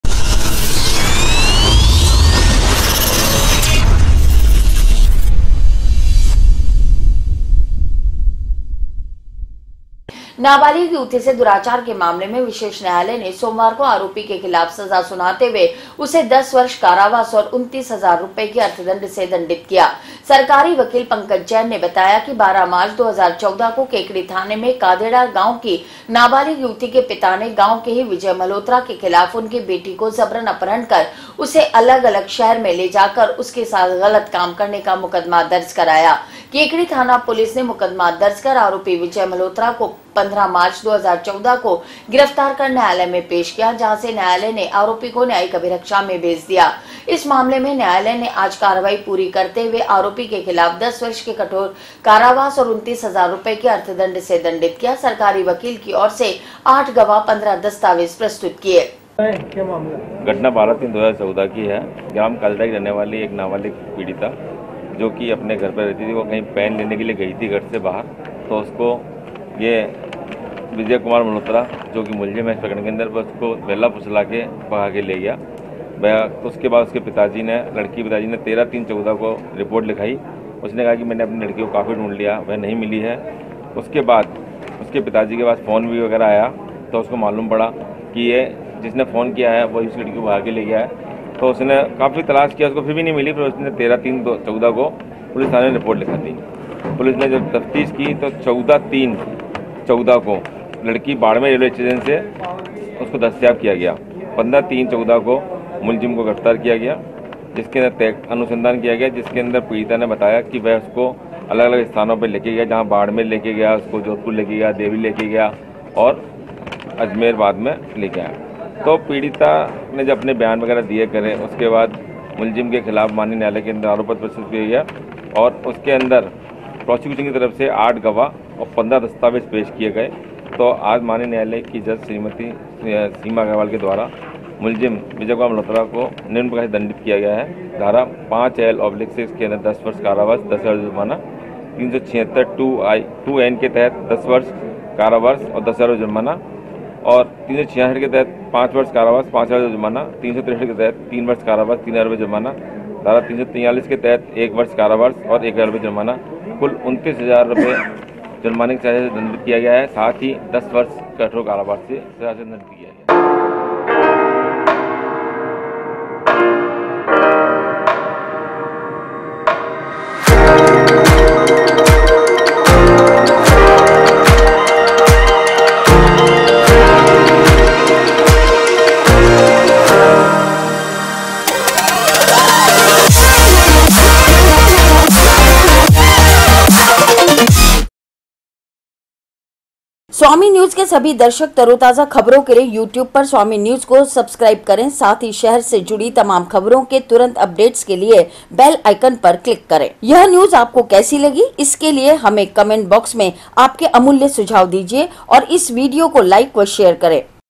नाबालिग से दुराचार के मामले में आरोपी को 10 साल कारावास की सजा 17 Dec 2018
नाबालिग युवती से दुराचार के मामले में विशेष न्यायालय ने सोमवार को आरोपी के खिलाफ सजा सुनाते हुए उसे 10 वर्ष कारावास और 29 हजार रूपये के अर्थदंड से दंडित किया।
सरकारी वकील पंकज जैन ने बताया कि 12 मार्च 2014 को केकड़ी थाने में कादेड़ा गांव की नाबालिक युवती के पिता ने गांव के ही विजय मल्होत्रा के खिलाफ उनकी बेटी को जबरन अपहरण कर उसे अलग-अलग शहर में ले जाकर उसके साथ गलत काम करने का मुकदमा दर्ज कराया। केकडी थाना पुलिस ने मुकदमा दर्ज कर आरोपी विजय मल्होत्रा को 15 मार्च 2014 को गिरफ्तार कर न्यायालय में पेश किया। जहां से न्यायालय ने आरोपी को न्यायिक अभिरक्षा में भेज दिया। इस मामले में न्यायालय ने आज कार्यवाही पूरी करते हुए आरोपी के खिलाफ 10 वर्ष के कठोर कारावास और 29 हजार रूपये के अर्थदंड से दंडित किया। सरकारी वकील की ओर से 8 गवाह 15 दस्तावेज प्रस्तुत किए गए।